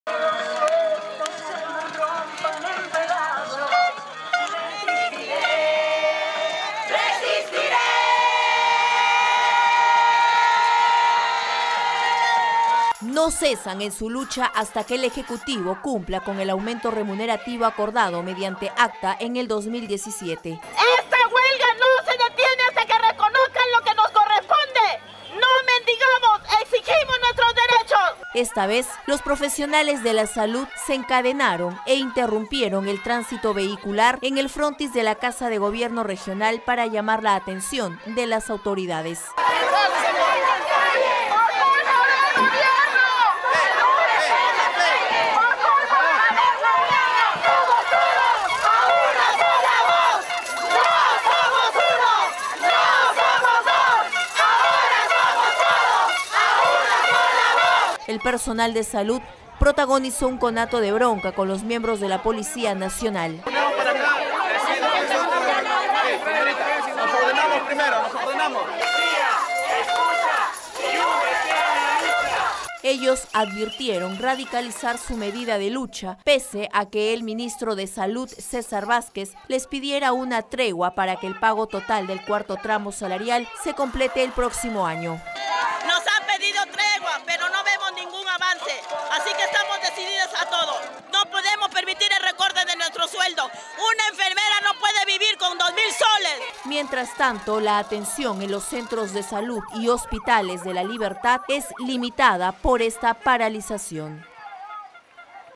Trujillo personal de salud se encadenan e interrumpen el tránsito
Los profesionales de la salud no dan tregua y continúan con su medida de lucha. Con el fin de llamar la atención de las autoridades, se encadenaron e interrumpieron el tránsito vehicular en el frontis de la casa del Gobierno Regional.

No cesan en su lucha hasta que el ejecutivo cumpla con el aumento remunerativo acordado mediante acta en el 2017.

Esta vez, los profesionales de la salud se encadenaron e interrumpieron el tránsito vehicular en el frontis de la casa de Gobierno Regional para llamar la atención de las autoridades.
El personal de salud protagonizó un conato de bronca con los miembros de la Policía Nacional.

Ellos advirtieron radicalizar su medida de lucha, pese a que el ministro de Salud les pidiera una tregua para que el pago total del cuarto tramo salarial se complete el próximo año.
Mientras tanto, la atención en los centros de salud y hospitales es limitada por esta paralización.